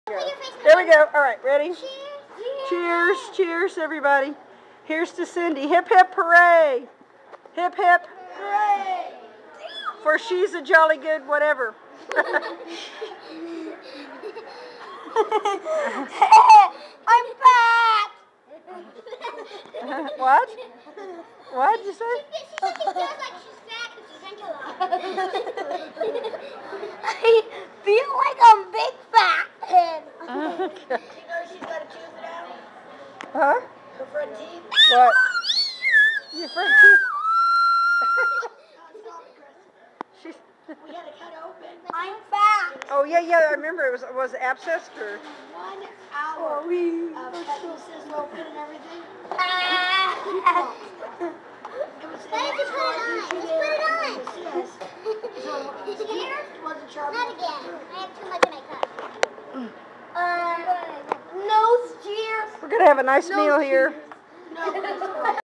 Cynthia's Eighth Birthday
The girls "toast" to Cindy's birthday at our favorite pizzeria, TAVO'S..